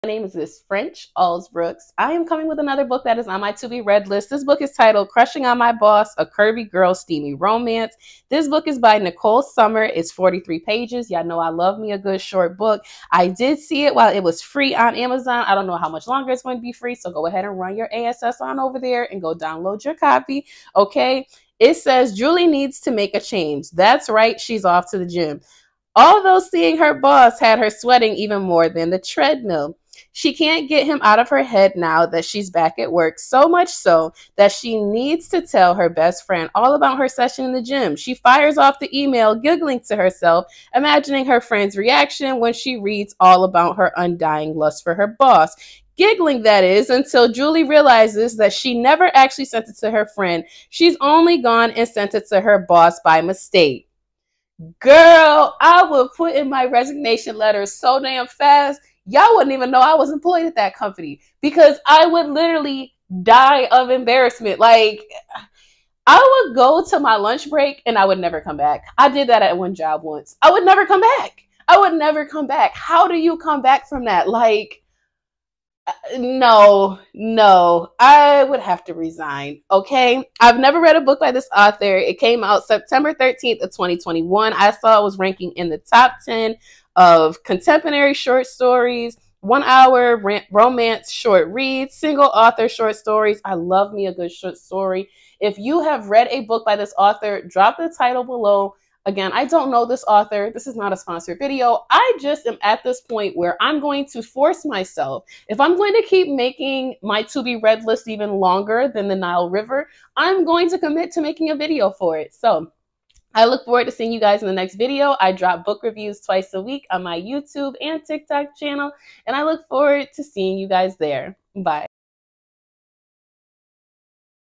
Now On My TBR| Crushing on my Boss: A Curvy Girl Steamy Romance
Nicole Summer
Crushing on my Boss: A Curvy Girl Steamy Romance
Book overview
Julie needs to make a change…

…that’s right, she’s off to the gym.

Although seeing her boss had her sweating even more than the treadmill!

She can’t get him out of her head now that she’s back at work. So much so, she just needs to tell her best friend all about her session in the gym. She fires off the email giggling to herself, imagining her friend’s reaction when she reads all about her undying lust for her boss. Giggling that is, until Julie realizes she never actually sent it to her friend. She’s only gone and sent it to her boss by mistake!

How will Julie recover from this?

Will she get fired?

How will her boss react?

You’ll love this short stand-alone, hot and steamy, office romance because it’s filled with enough conflict to keep you guessing the outcome until the end. Comes with HEA and No Cliffhangers!